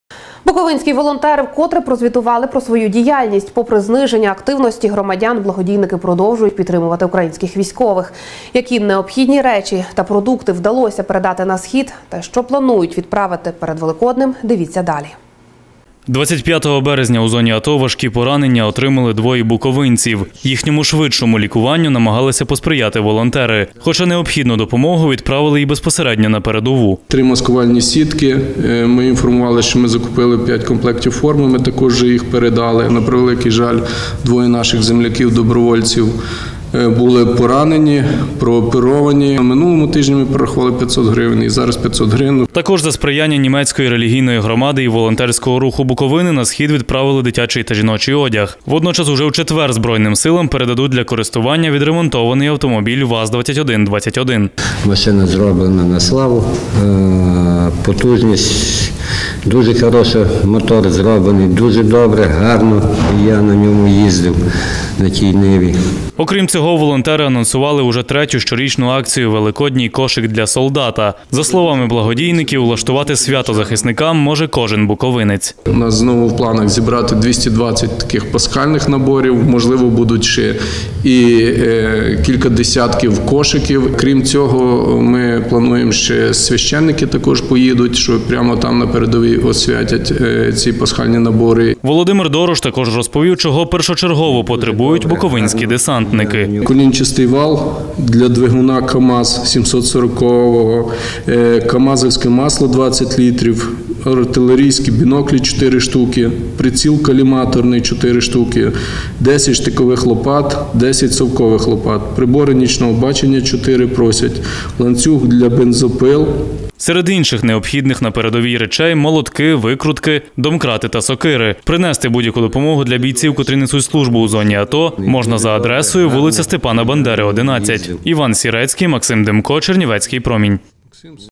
Волонтери просять буковинців долучитися до акції «Великодній кошик для солдата»
Допомогти нашим захисникам відчути свято за сотні кілометрів від рідного дому може кожен.
Відео телеканалу Чернівецький Промінь (с) 170404